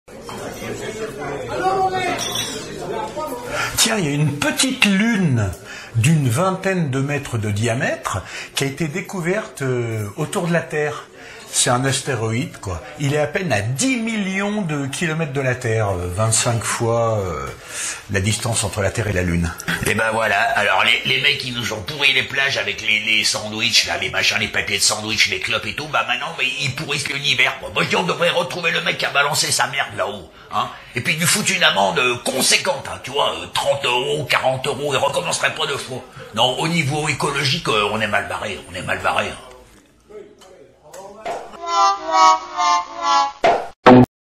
AU BISTRO - Une mini Lune autour de la Terre
Aujourd'hui au bistro, on parle de cette découverte récente dans notre proche univers. Une mini Lune !

Merci d'avance  ;-)